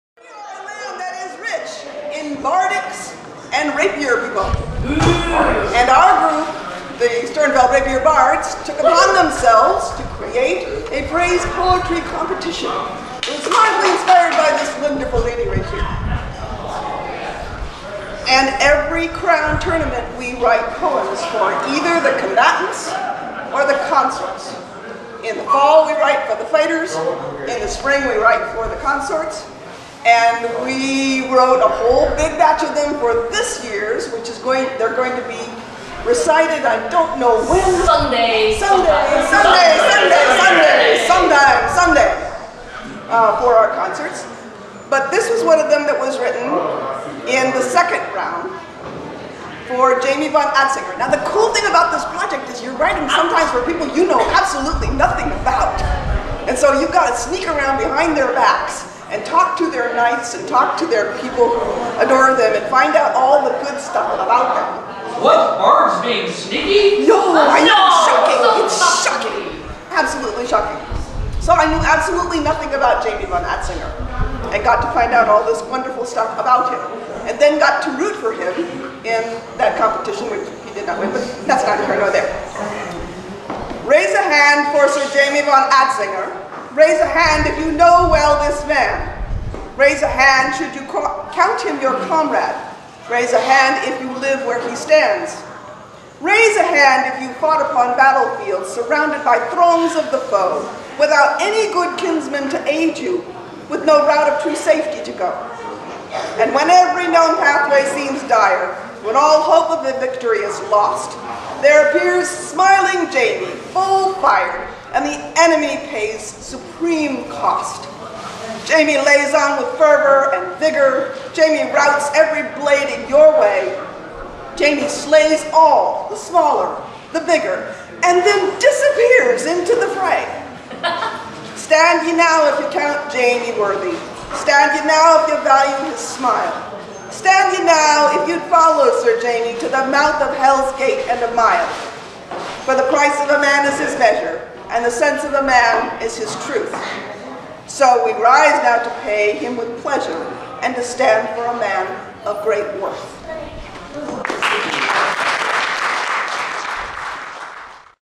Praise Poem for Jamie von Anzinger
Líadan Liathán. The Sternveldt Rapier Bards have started writing praise poems for the combatants (Fall) and the consorts (Spring) for Midrealm Crown competitors. This is one of them. Recorded in the Tavern at Middle Kingdom 50 Year Anniversary, the weekend of May 24-27, 2019 in the Barony of Sternveldt (in Danville, Indiana, at the Hendricks County 4-H Fairground.)